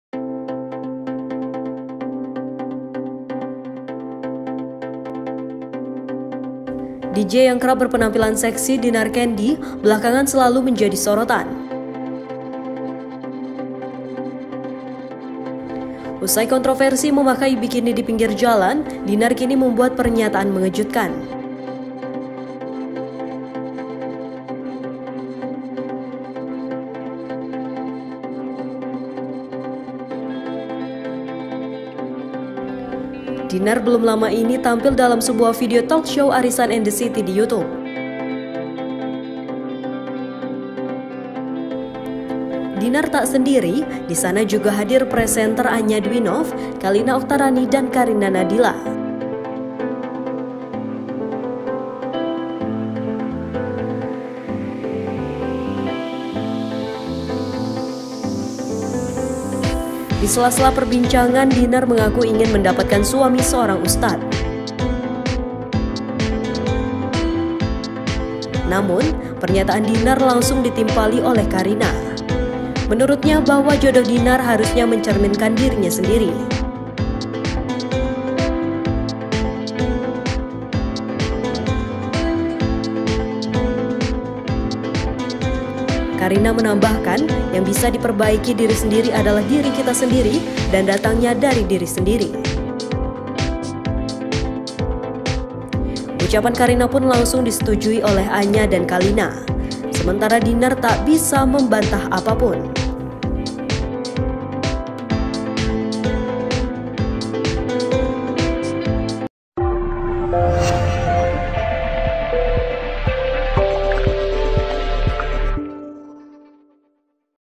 HASRAT DINAR CANDY INGIN PUNYA SUAMI USTAD UNTUK PERBAIKI DIRI
DJ yang kerap berpenampilan seksi, Dinar Candy
belakangan selalu menjadi sorotan.

Usai kontroversi memakai bikini di pinggir jalan,
Dinar kini membuat pernyataan mengejutkan.

Dinar belum lama ini tampil dalam sebuah video
talkshow Arisan and The City di Youtube.